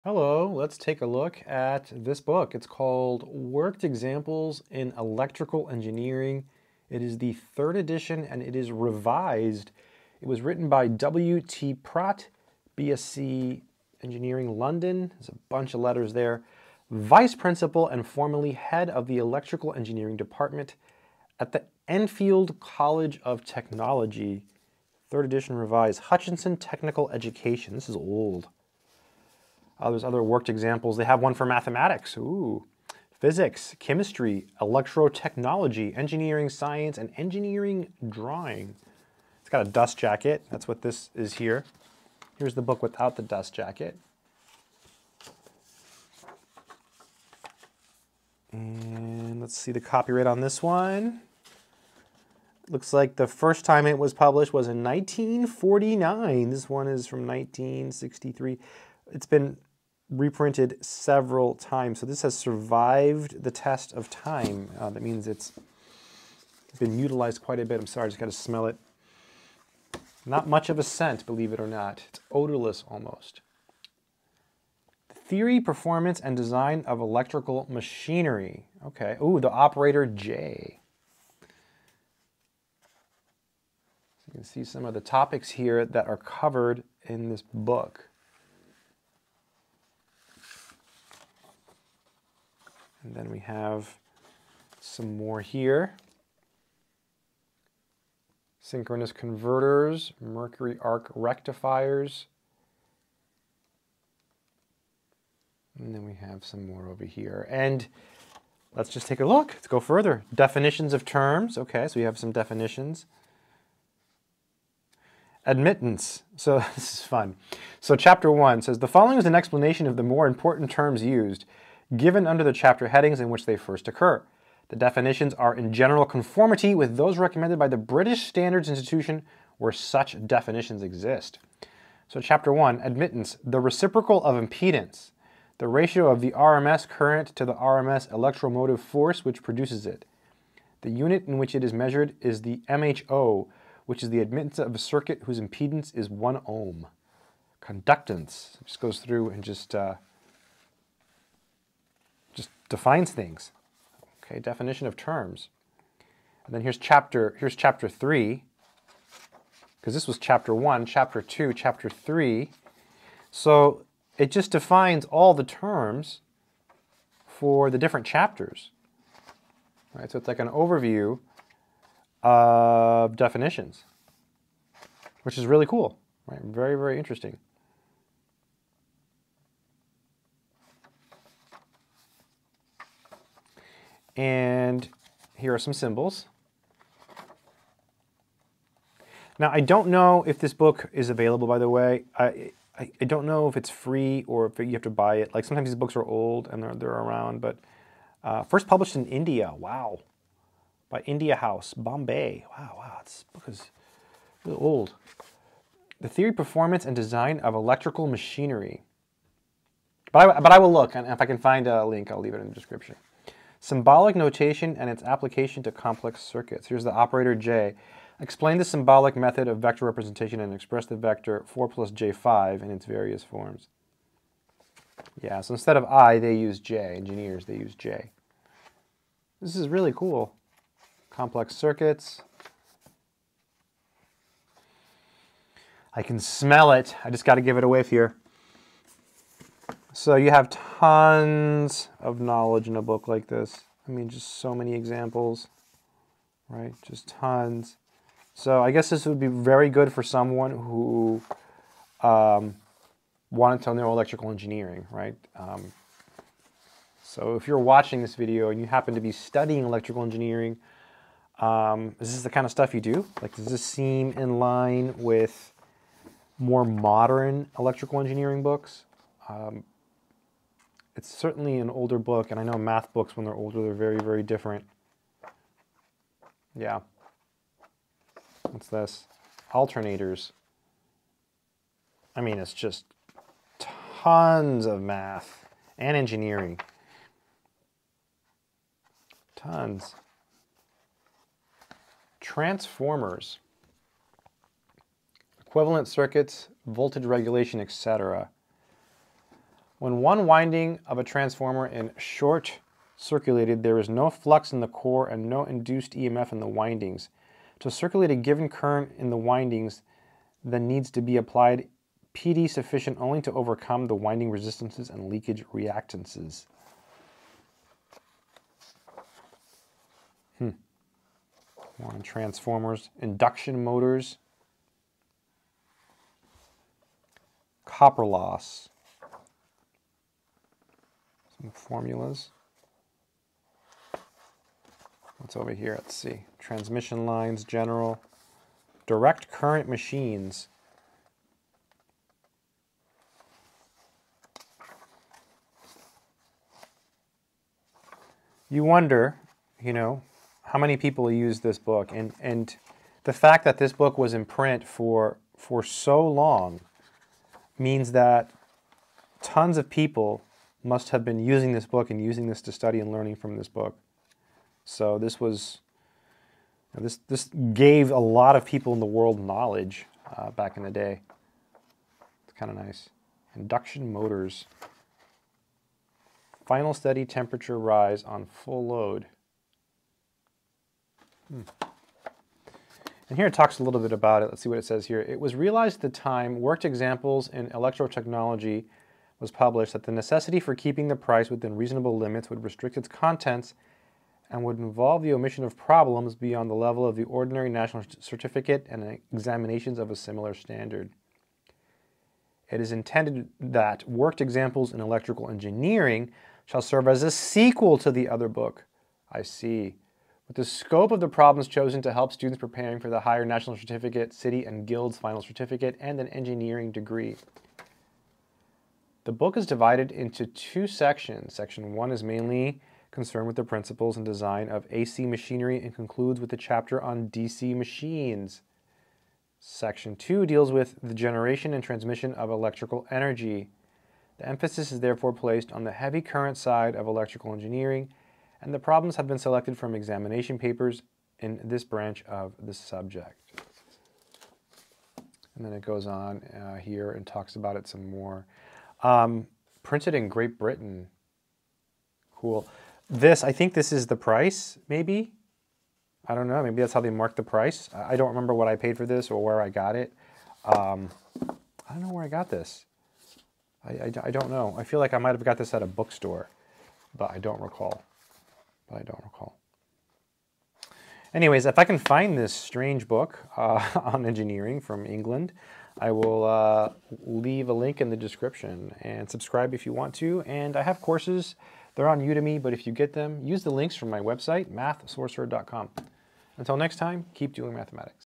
Hardcore Electrical Engineering Book
This is an old book titled Worked Examples in Electrical Engineering. It was written by W.T. Pratt.